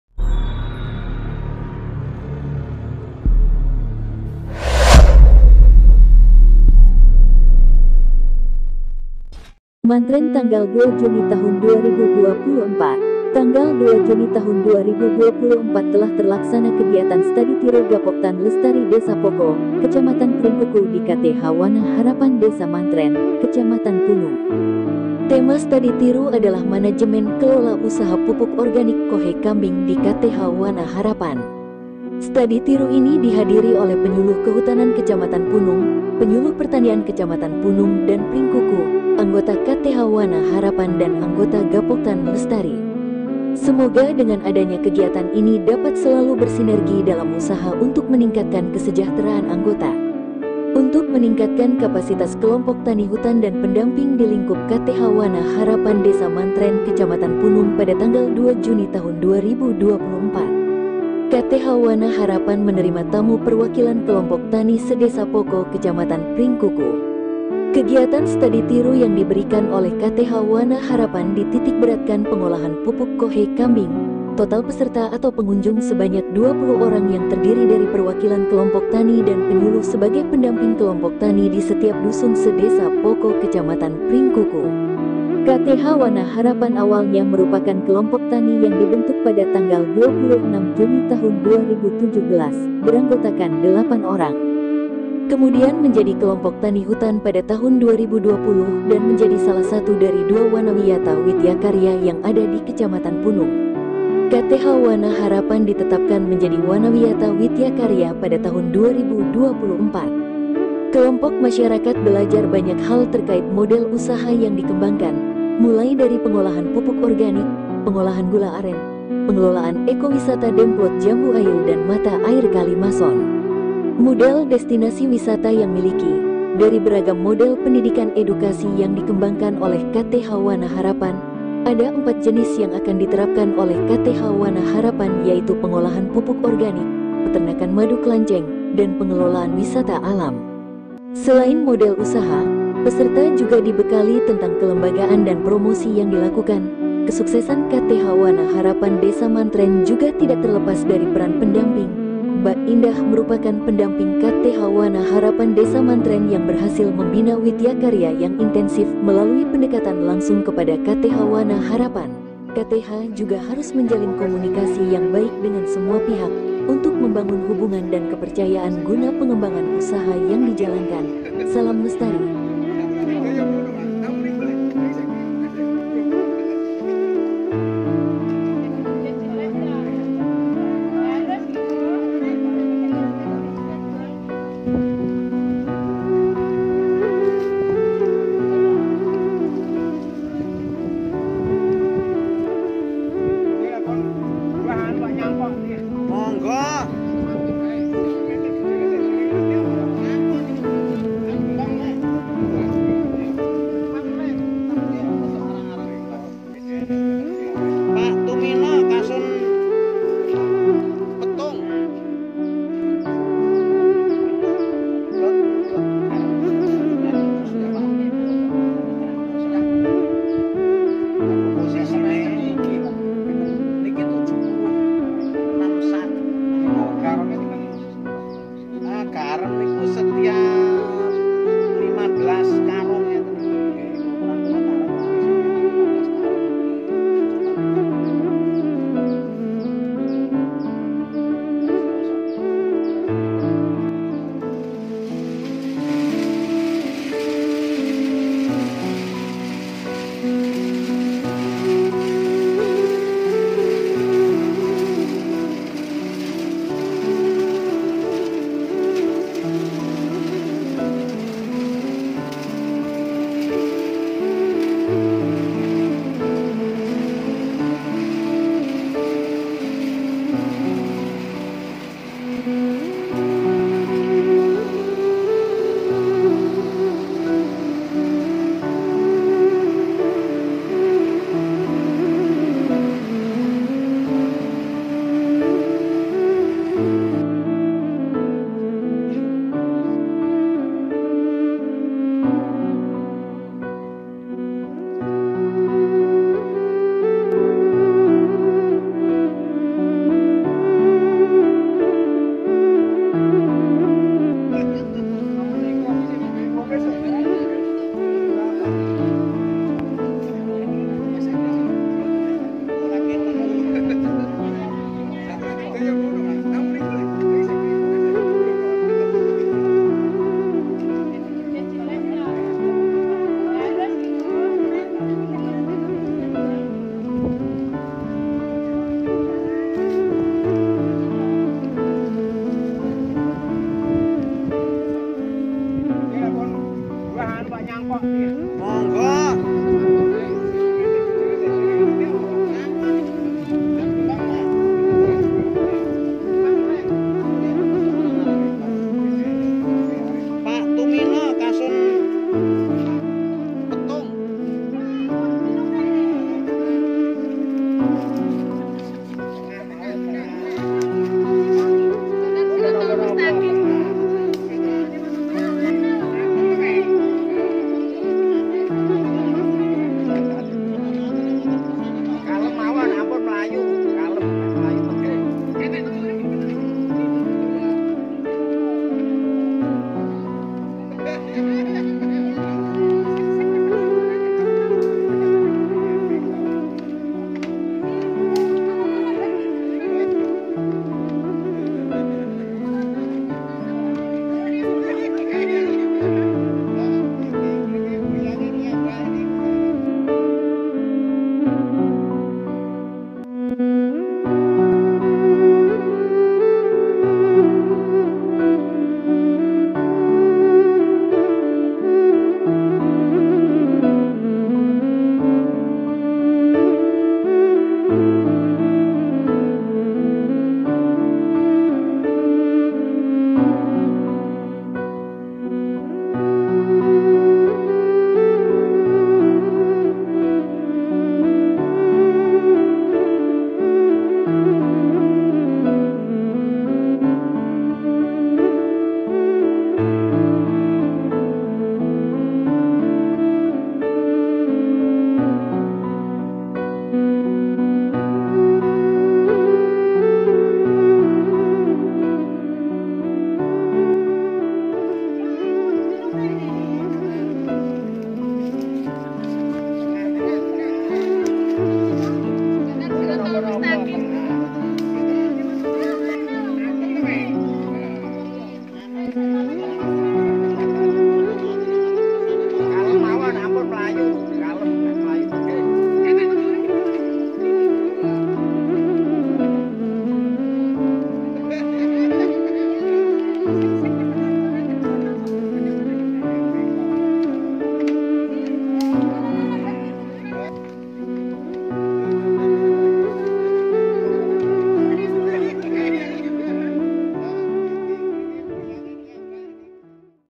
Study Tiru Kelompok Tani Lestari Poko di KTH Wana Harapan Desa Mantren 2024
Tanggal 02 Juni 2024 telah terlaksana kegiatan Study Tiru Gapoktan Lestari Desa Poko, Kecamatan Pringkuku di KTH Wana Harapan Desa Mantren, Kecamatan Punung. Tema Study Tiru adalah managemen kelola usaha pupuk organik kohe kambing di KTH Wana Harapan. Study Tiru ini dihadiri oleh Penyuluh Kehutanan Kec. Punung, Penyuluh Pertanian Kec. Punung dan Pringkuku, Anggota KTH Wana Harapan dan Anggota Gapoktan Lestari. Semoga dengan adanya kegiatan ini dapat selalu bersinergi dalam usaha untuk meningkatkan kesejahteraan anggota.